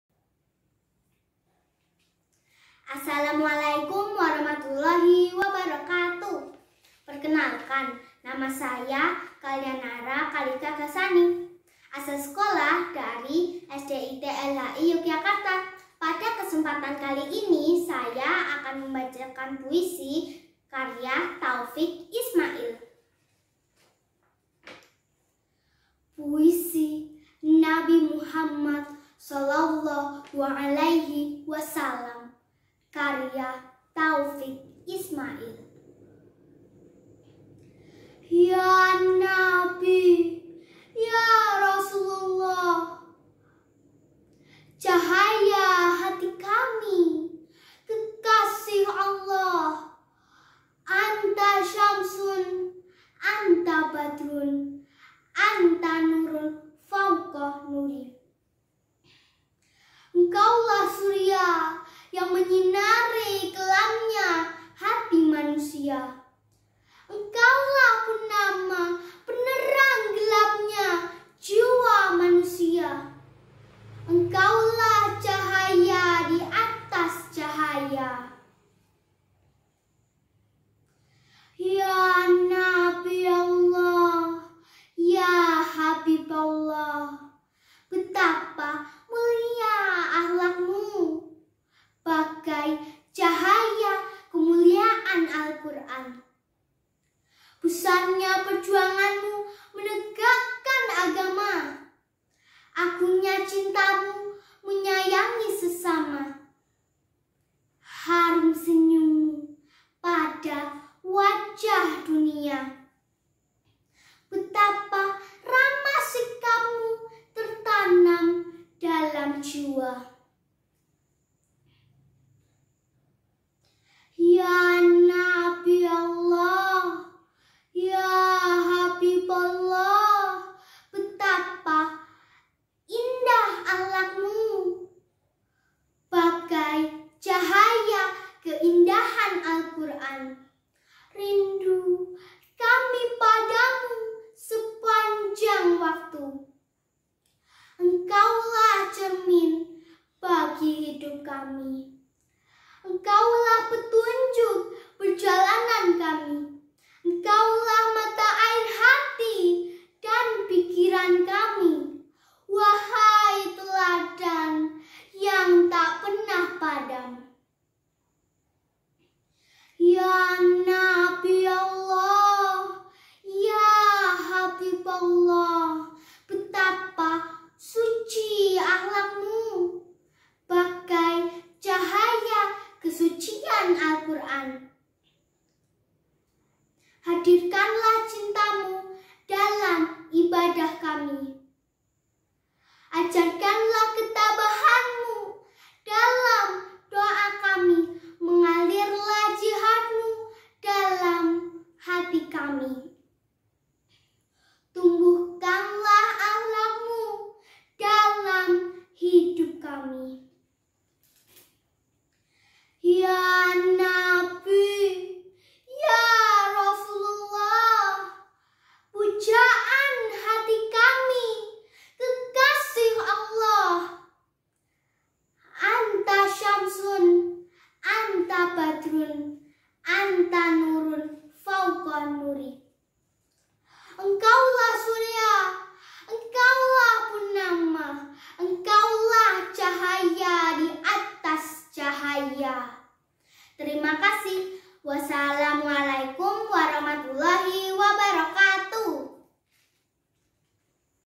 089 Kalyanara Kalika Baca Puisi Mifest Miftahunnajah
MIFEST (Miftahunnajah Islamic Festival) 2021 adalah event lomba Tingkat Nasional yang diselenggarakan oleh Pondok Pesantren Modern Miftahunnajah Yogyakarta.
Terdiri atas 3 cabang lomba yaitu Tartil, Dai Daiyyah, dan Baca Puisi.

Selamat berkreativitas dan berjuang
Salam Sukses